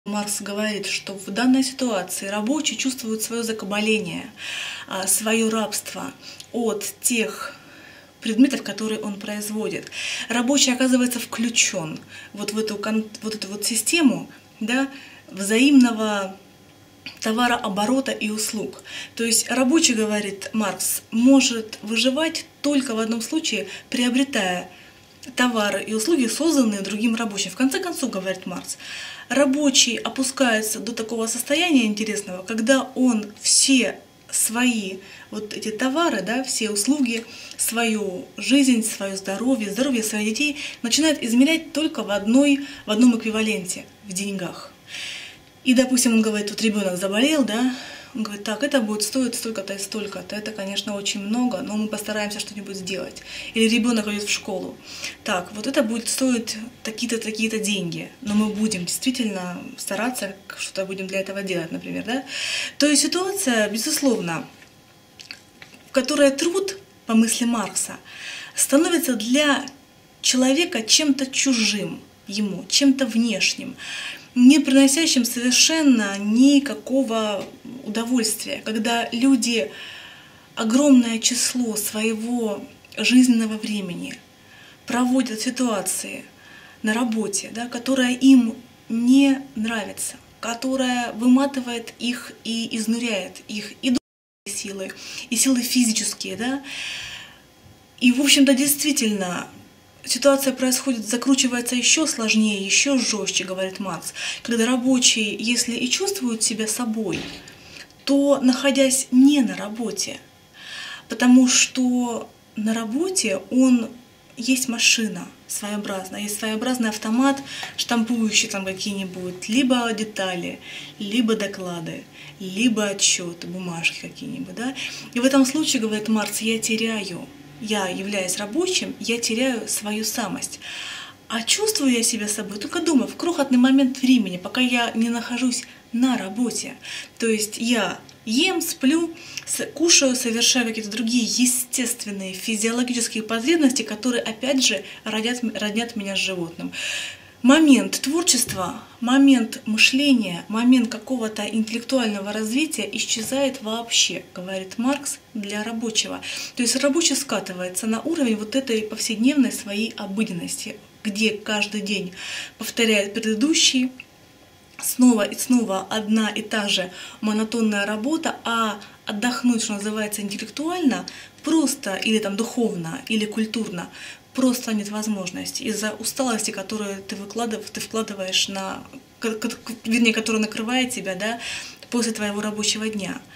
Почему работа нам в тягость? Елена Павлова
Отчуждение от труда по Карлу Марксу.
Рассказывает Елена Павлова.